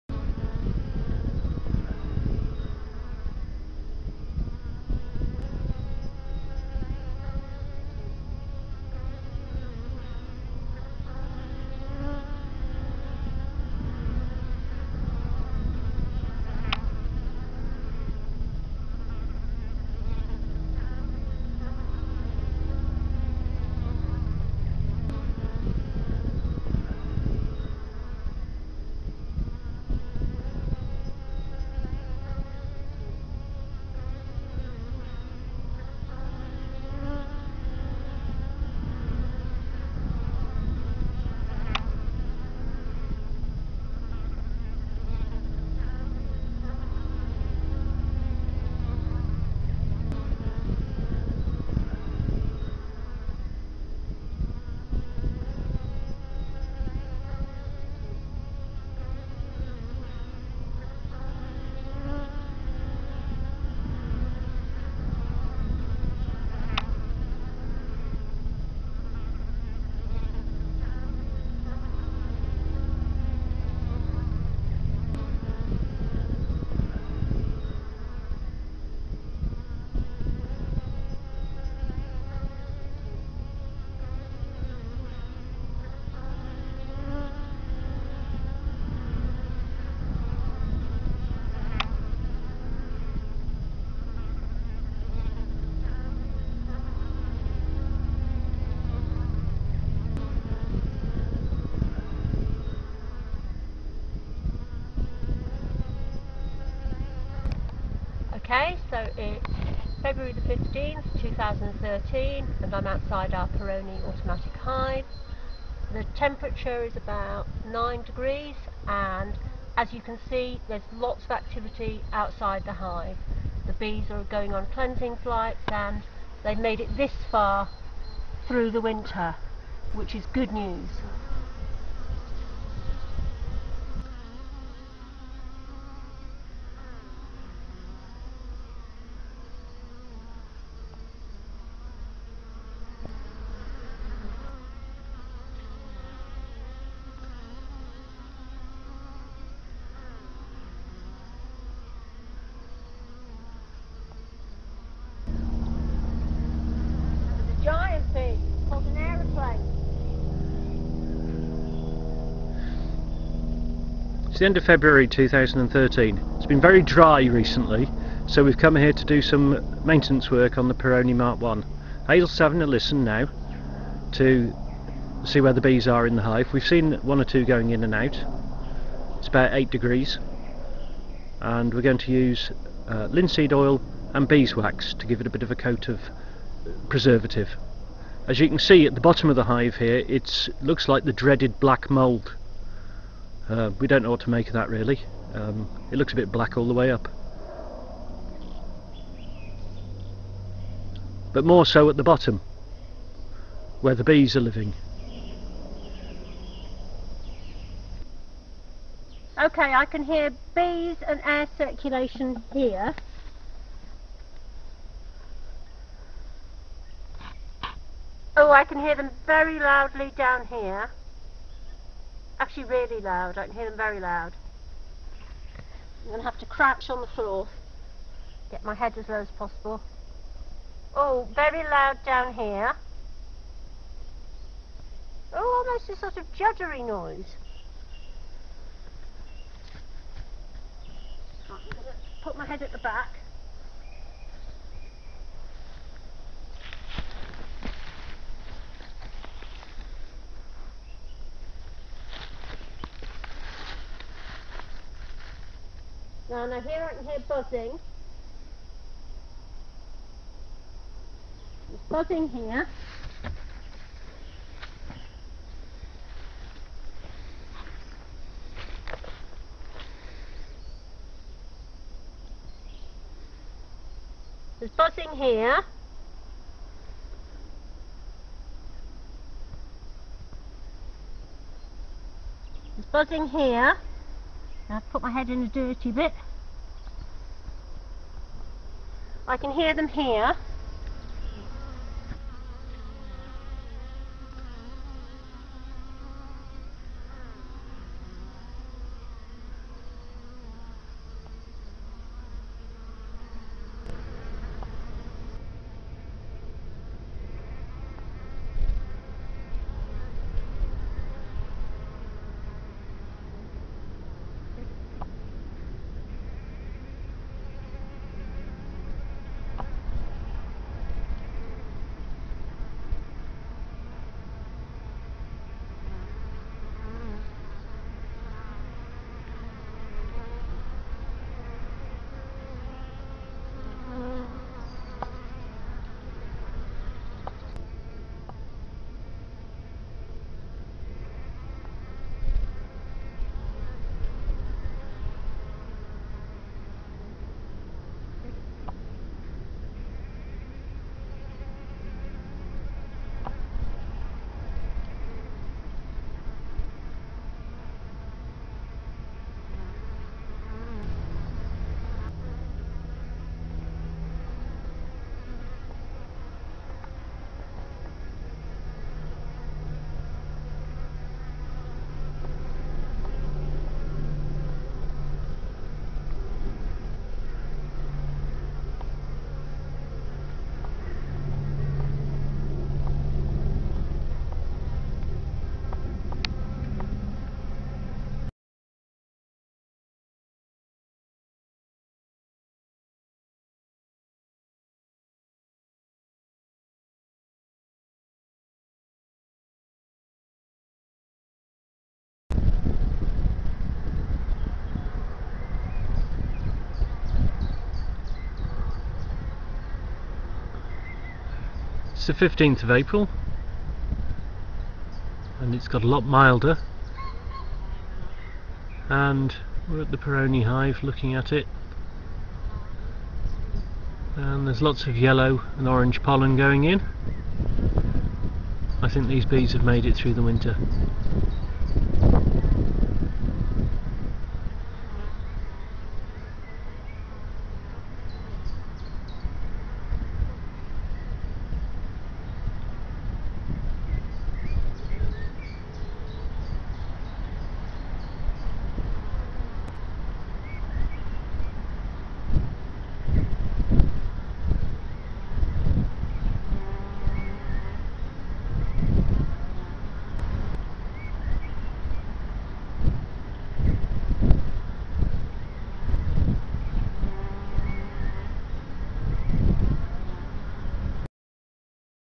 Perone Hive Experiment Part 2
Part 2 of our UK experiment with native black bees, 'Apis mellifera mellifera' & a Perone Automatic Hive Mk 1 (colmena Perone UK).